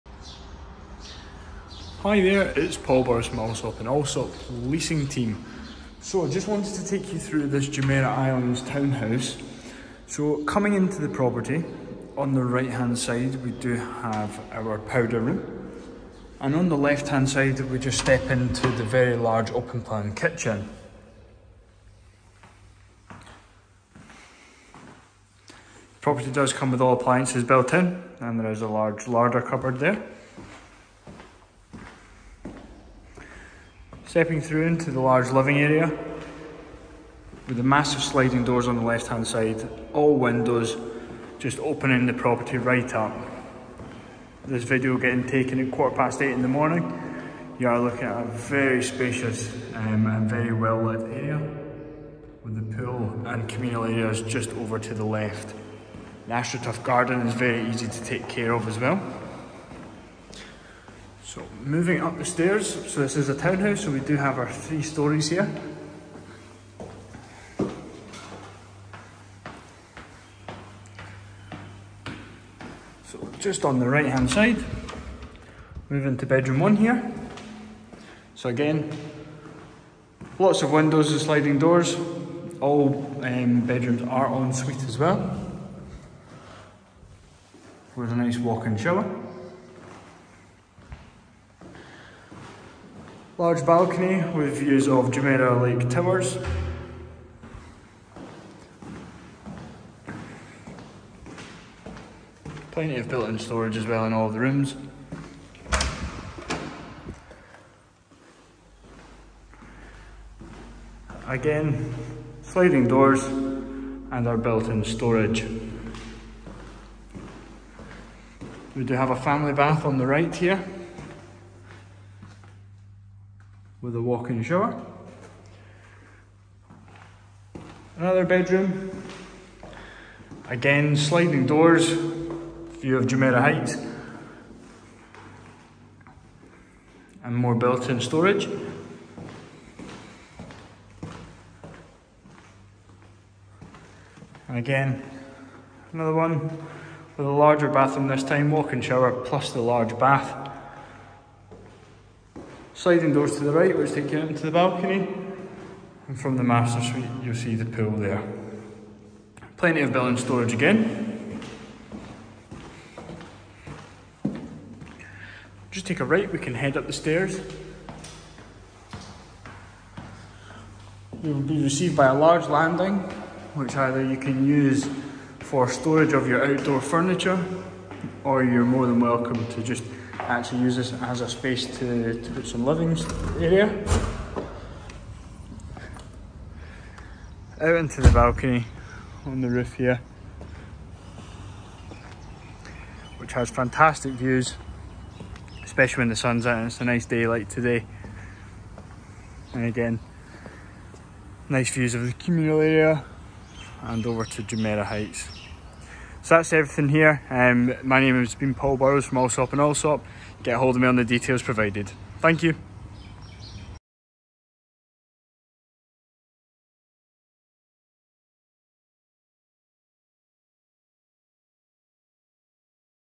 Vacant now: 4-BR with landscaped garden in Jumeirah Islands!
Four Bedroom | Well Maintained | Vacant

- 4 Bedroom + Maid’s Room
- Next to Community pool and Park
- size: 2,322.74 sq/ft
- Spacious open concept living and dining room
- Closed Modern Fitted kitchen style
- Maid’s Room en-suite on the ground floor
- 4 Bedroom are on the upper level, En-suite & built in wardrobe
- Living and dining room have access to the garden
- Matured landscape garden
- 2 parking spaces
- Well maintained Villa
- Vacant and ready to move-in